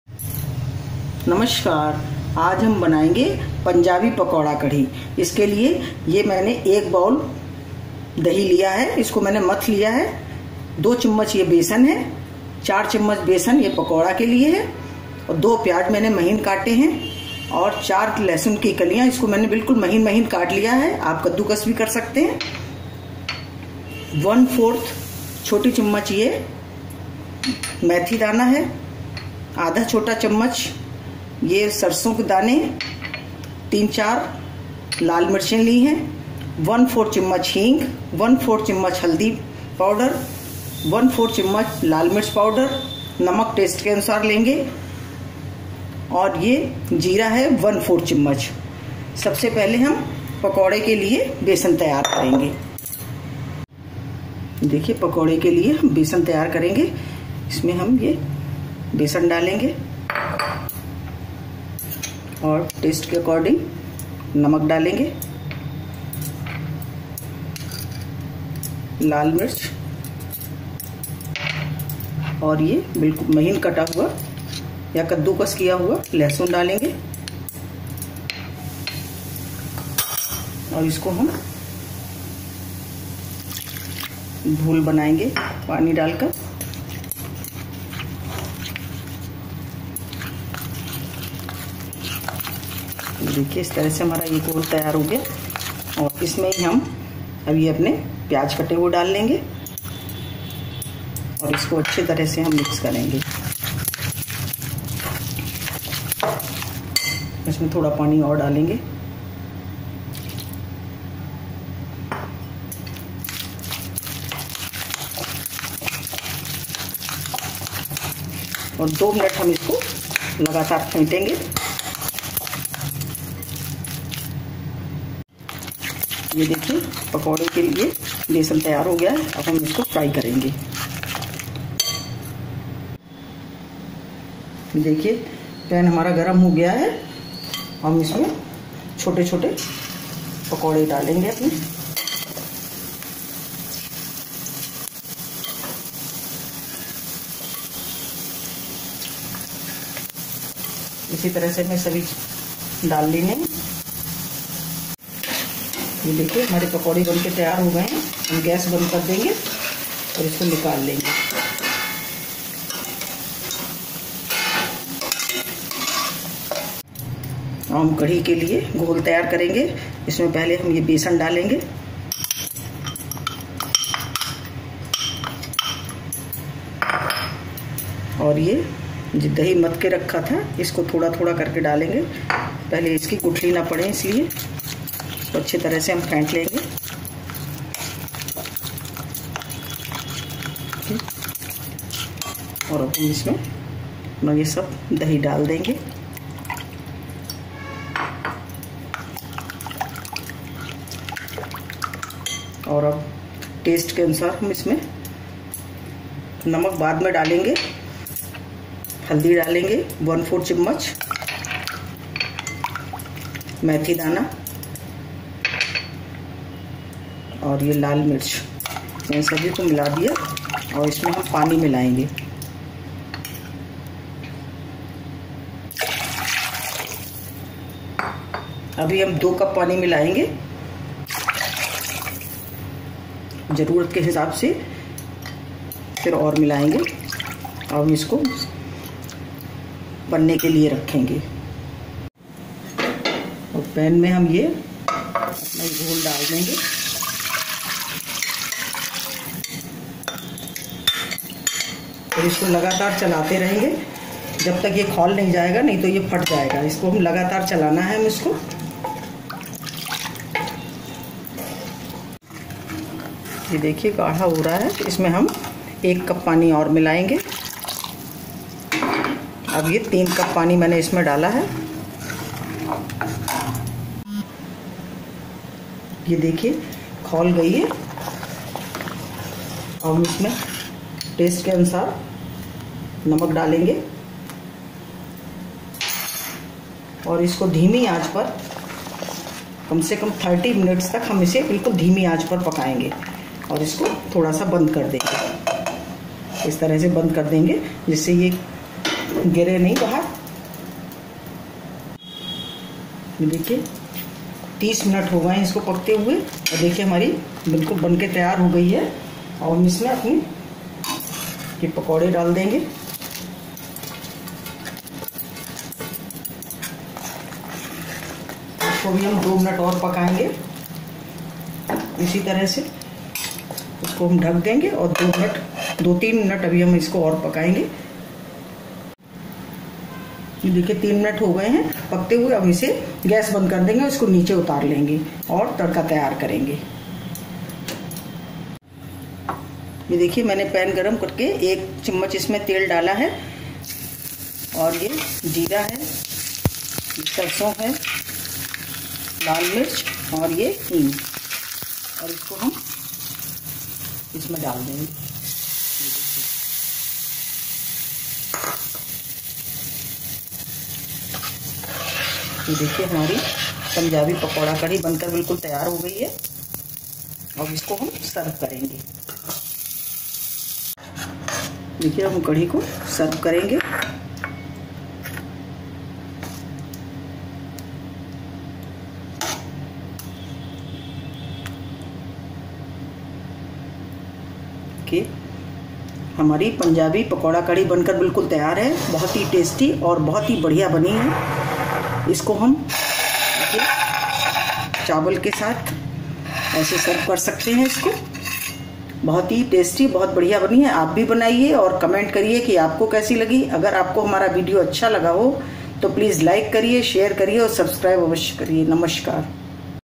पंजाबी पकौड़ा कढी बनाये आसान तरीके से | Panjabi Pakoda Kadi Recipe
पंजाबी पकौड़ा कढी बनाये आसान तरीके से
Panjabi Pakoda Kadi Recipe

सामग्री:

दही 250 gm
पकौड़े के लिये बेसन 60 gm
घोल के लिए बेसन 30 gm
पानी घोल के लिए 800 gm
लहसुन की कलियाँ 4 पीस
2 प्याज
1/2 छोटा च. मैथी दाना
1 छोटा च.सरसों दाने
1/4 छोटा च.जीरा
1/2 छोटा च.लाल मिर्च पाउडर
1/4 छोटा च.हीग
नमक
3 सूखी लाल मिर्च
तेल तलने के लिये
1/4 च. हल्दी पाउडर

Ingredients:

Gram flour for dumplings 60 gm
Gram flour for solution 30 gm
800 gm water for solution
Garlic buds 4 pieces
2 onions
1/2 tsp fenugreek seeds
1 tsp mustard seeds
1/4 teaspoon cumin seeds
1/2 teaspoon red chilli powder
1/4 tsp asafoetida
Salt
3 dry red chillies
To fry oil
1/4 teaspoon turmeric powder